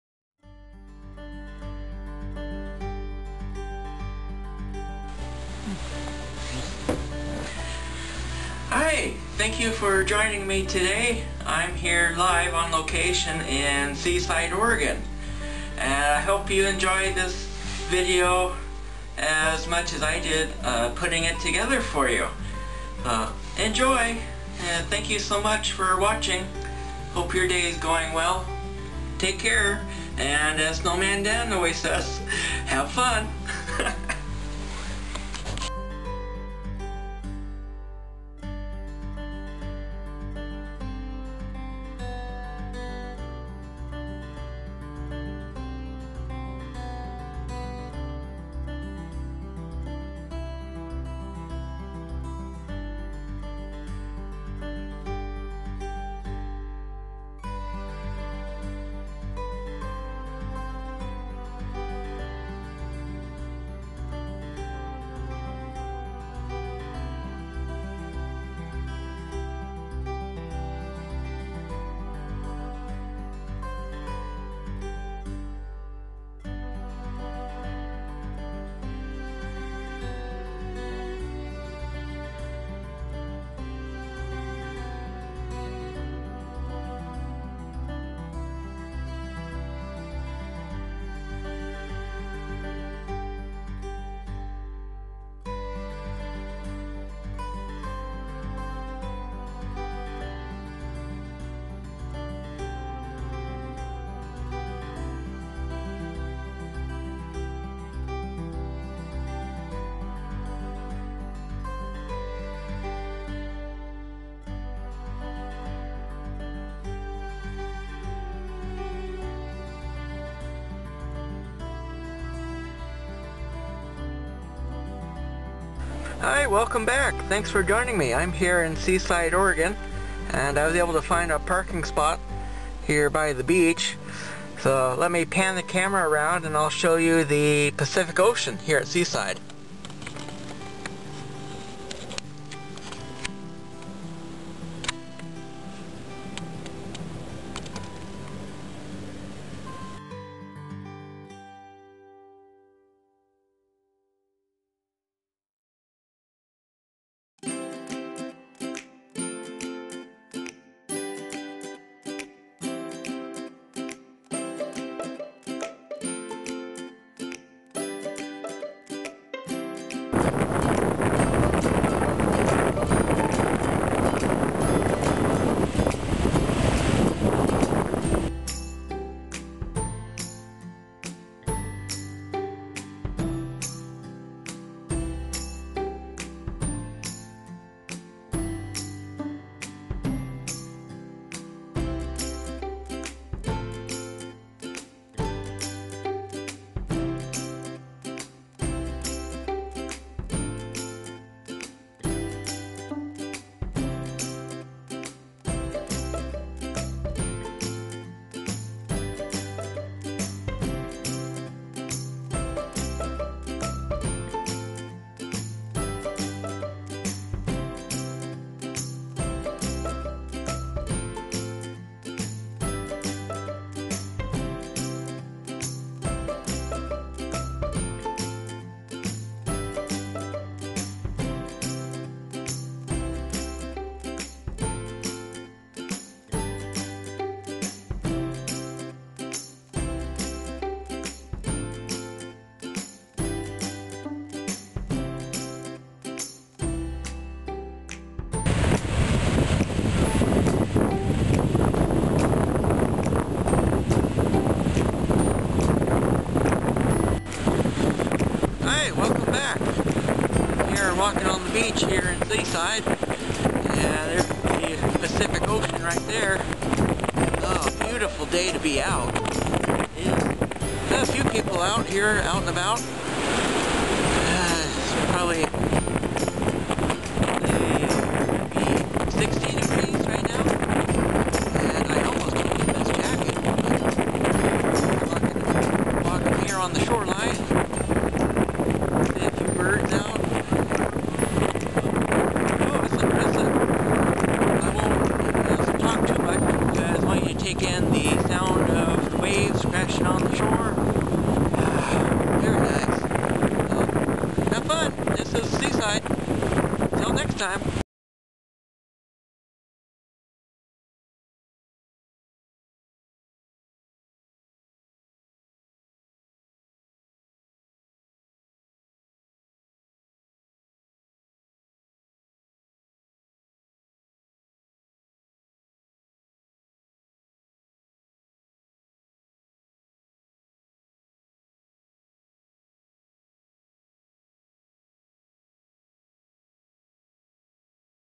Seaside Part One
Filmed on April 29th and 30th, 2019 in Seaside, OR. Seaside is located in Clatsop County along Highway 101. Enjoy! Please check this area out if you are ever in northwestern Oregon.

Fritz film footage and photos were assembled using Canon Zoom Edge editing software. Good job.